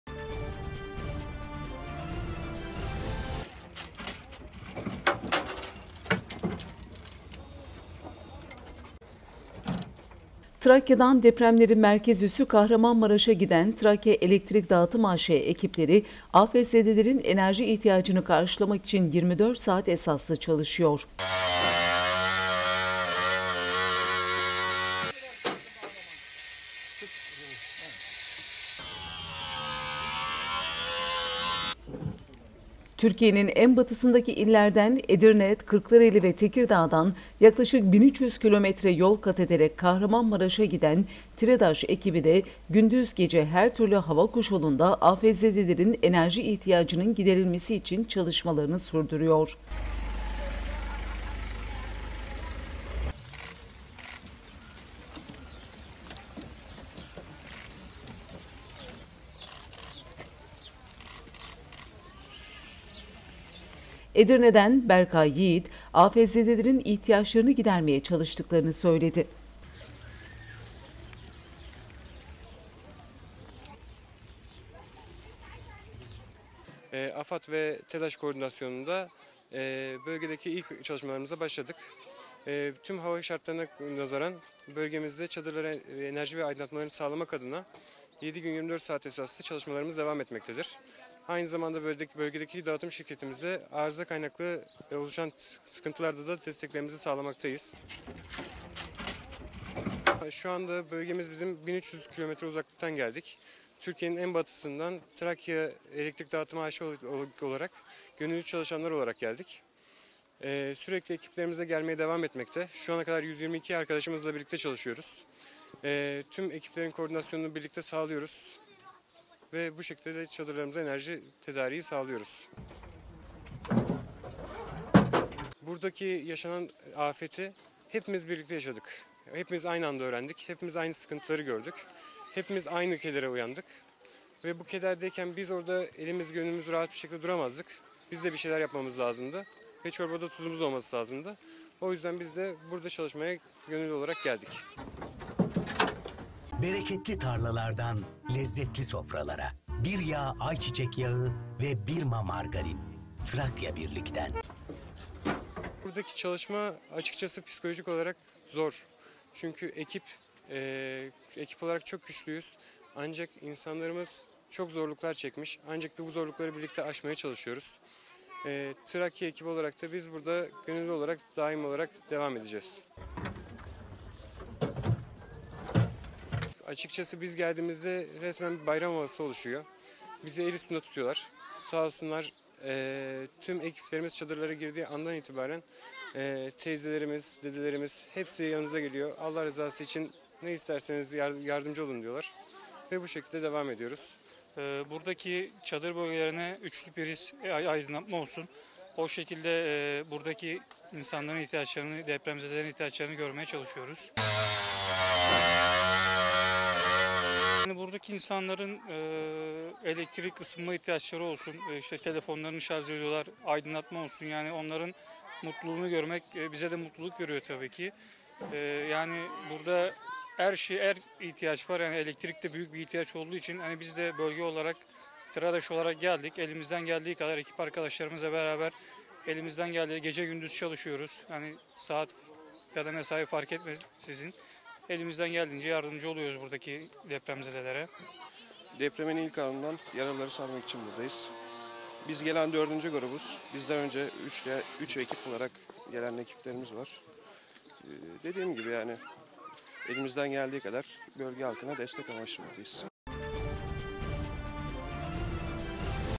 Enerji timleri afetzedeler için görev başında
TREDAŞ ekipleri afet bölgesinde
Trakya'dan depremlerin merkez üssü Kahramanmaraş'a gelen Trakya Elektrik Dağıtım AŞ (TREDAŞ) ekipleri, afetzedelerin enerji ihtiyacını karşılamak için 24 saat esaslı çalışıyor.
Pazarcık ve Elbistan ilçeleri merkezli 6 Şubat'ta meydana gelen 7,7 ve 7,6 büyüklüğündeki depremlerin etkilediği illerde birçok binanın yıkılması, alt ve üstyapının zarar görmesi nedeniyle enerji kesintileri yaşandı. Sorunlarının çözülmesi amacıyla ülke genelindeki elektrik dağıtım şirketlerinin ekipleri bölgede görev aldı.
Türkiye'nin en batısındaki illerden Edirne, Kırklareli ve Tekirdağ'dan, yaklaşık 1300 kilometre yol kat ederek Kahramanmaraş'a gelen TREDAŞ ekibi de gündüz gece her türlü hava koşulunda afetzedelerin enerji ihtiyacının giderilmesi için çalışmalarını sürdürüyor.
Çadır kentlerde aydınlatma sistemlerinin kurulması ve bakımı görevini üstelenen ekipler, aynı zamanda çadırlarda kullanılan lamba, ısıtıcı ve elektronik aletler için enerji akışını sağlıyor.
- "Günlük ortalama 300 çadıra ulaşıyoruz. Yaklaşık 7 bin çadıra hizmet verdik"
Tekirdağ'ın Çorlu ilçesinden gelen elektrik mühendisi Hakan Dedeoğlu, afetin ardından gönüllü olarak ekiplerle kente ulaştıklarını söyledi.
Çadır kentlere enerji verme ve aydınlatma çalışmalarını 24 saat esaslı yürüttüklerini anlatan Dedeoğlu, "Biz 1300 kilometre uzaktan geldik. Türkiye'nin en batısından Trakya Elektrik Dağıtım A.Ş. olarak gönüllü çalışanlarıyla geldik. Ekiplerimiz gelmeye devam ediyor. 122 arkadaşımızla çalışıyoruz. Çadırlarımıza enerji tedarik ediyoruz." diye konuştu.
Dedeoğlu, yaşanan afetin çok büyük olduğunu vurgulayarak, "Bizim de bir şeyler yapmamız lazımdı. O yüzden buraya gönüllü olarak geldik." dedi.
Depremzede vatandaşlara yardım etmenin gururunu yaşadıklarını dile getiren Dedeoğlu, psikolojik açıdan bölgede çalışmanın zor olduğunu ancak zorlukları vatandaşlarla aşmaya çalıştıklarını ifade etti.
Dedeoğlu, gönüllü çalışmaya devam edeceklerine değinerek, şunları kaydetti:
"Geldiğimizde bayram havası oluşuyor, bizi el üstünde tutuyorlar. Sağ olsunlar. Ekiplerimiz çadırlara girdiği andan itibaren teyzelerimiz, dedelerimiz yanımıza geliyor, 'Allah rızası için ne isterseniz yardımcı olalım.' diyorlar. Burada ne kadar kalmamız gerekiyorsa kalacağız. Günlük ortalama 300 çadıra ulaşıyoruz. Yaklaşık 7 bin çadıra hizmet verdik."
- "Onların mutluluğunu görmek bizlere de mutluluk veriyor"
Edirne'den gelen Berkay Yiğit de afetzedelerin ihtiyaçlarını gidermeye çalıştıklarını aktardı.
Herkesin elektriğe ihtiyacının olduğuna işaret eden Yiğit, "İnsanların ısınma ihtiyaçları var, telefonlarını şarj ediyorlar. Onların mutluluğunu görmek bizlere de mutluluk veriyor tabii ki. Elektriğe büyük ihtiyaç olduğu için TREDAŞ olarak geldik, gece gündüz demeden çalışıyoruz." şeklinde konuştu.
Tekirdağ'dan gelen Eyüp Tosun, yaraları sarmak için bölgede bulunduklarını dile getirdi.
Her çadıra enerji sağladıklarını anlatan Tosun, "Onların acısını paylaşıyoruz. Elden ne gelirse yapmaya çalışıyoruz. Bir gün gelecek ve görev süremiz dolacak, bizler evlerimize döneceğiz ama aklımız hep burada kalacak." ifadesini kullandı.(aa)